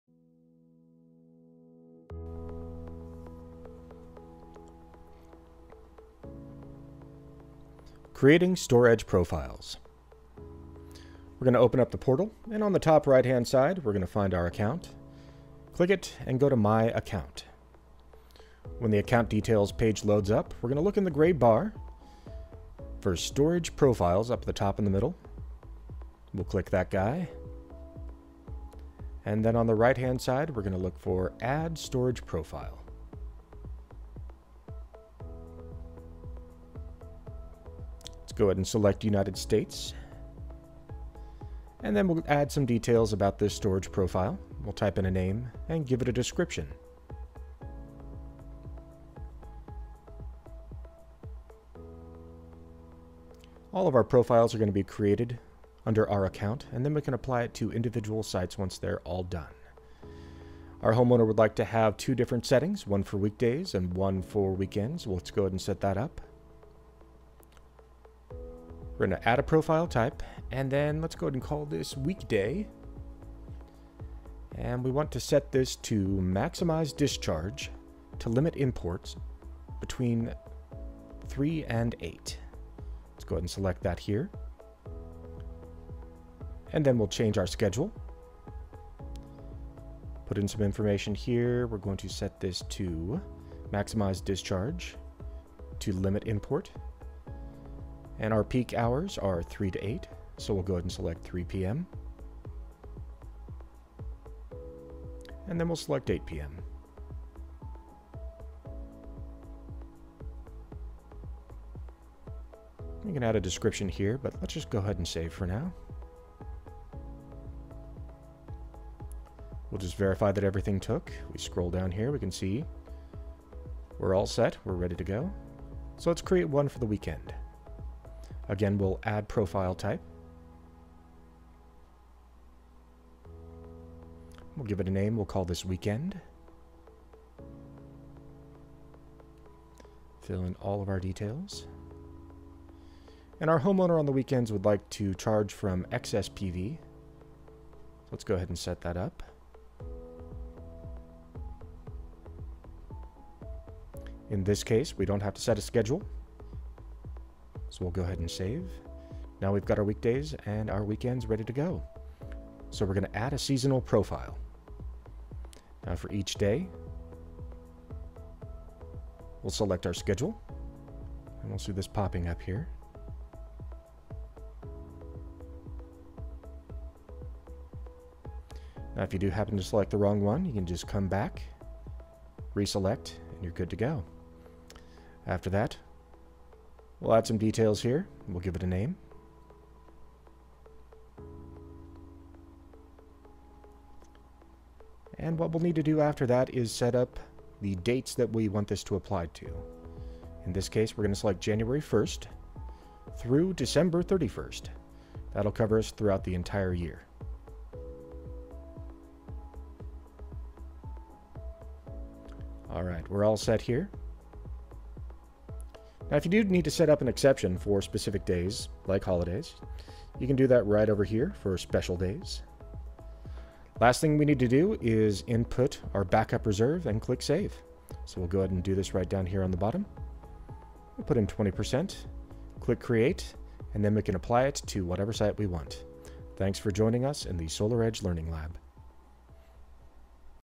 Creating a StorEdge Profile in the SolarEdge Monitoring Platform | International
Creating an account-based StorEdge profile demo in the SolarEdge monitoring portal. Profiles can be applied to individual sites after creation.